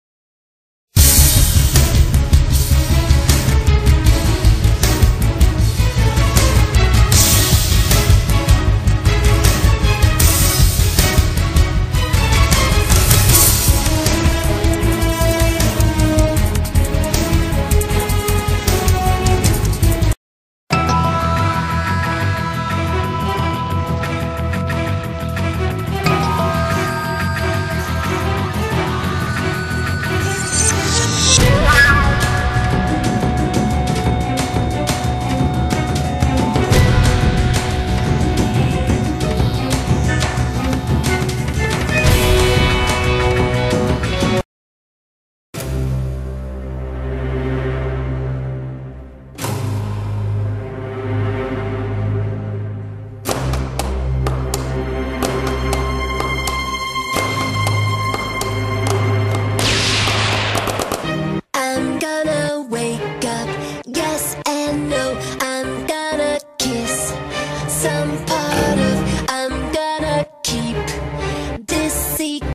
metal gear stickmen(trailer)
a new movie ime making coming probably about march 2009 anyone who subscribes will get sneak preview messages.